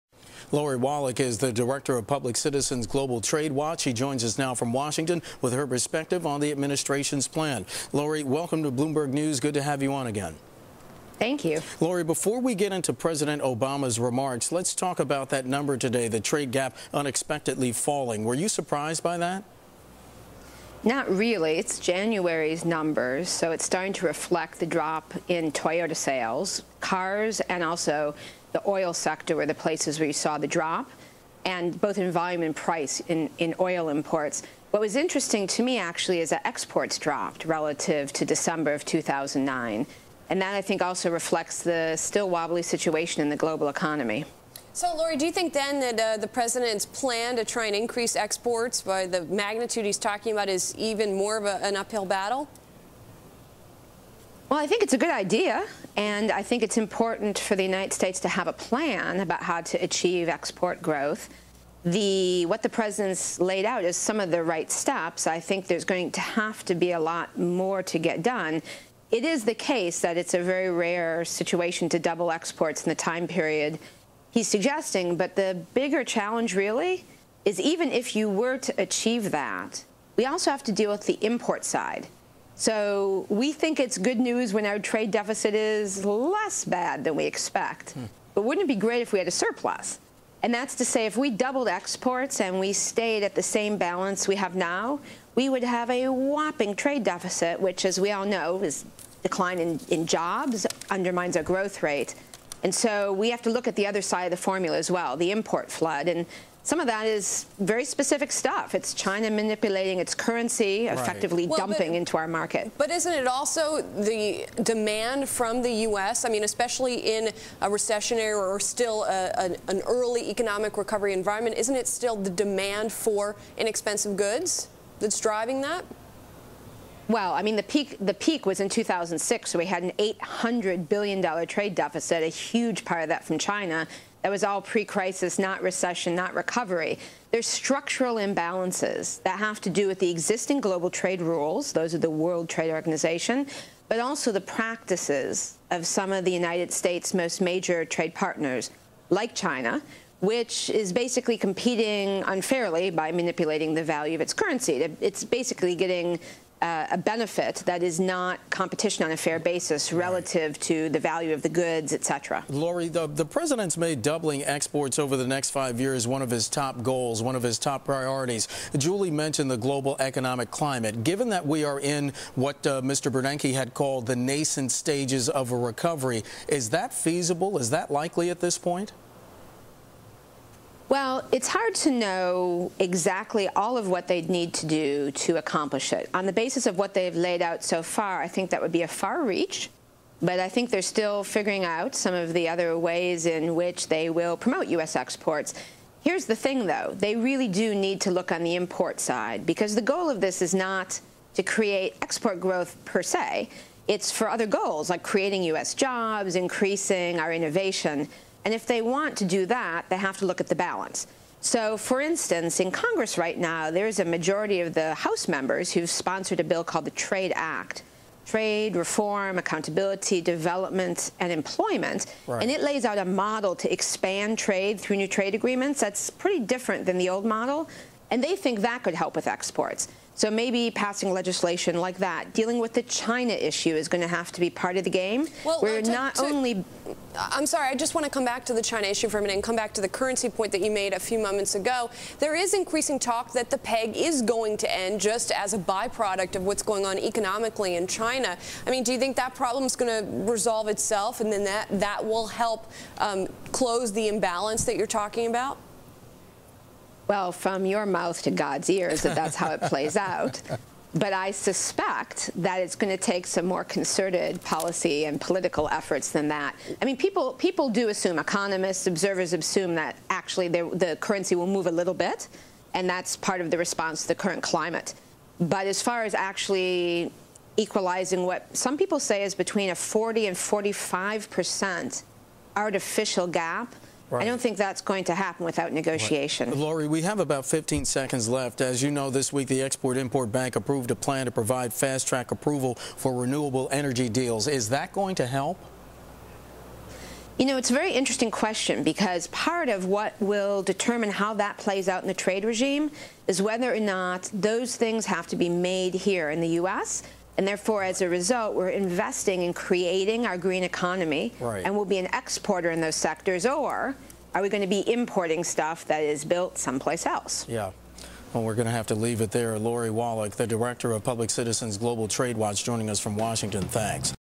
Wallach Says Export Data Shows `Wobbly' Global Economy: Video
March 11 (Bloomberg) -- Lori Wallach, president of Global Trade Watch, talks with Bloomberg's Mark Crumpton and Julie Hyman about the U.S. trade data and policies.
     The trade gap decreased 6.6 percent to $37.3 billion in January from a revised $39.9 billion in December as Americans imported the fewest barrels of crude oil in a decade, Commerce Department figures showed today in Washington. (Source: Bloomberg)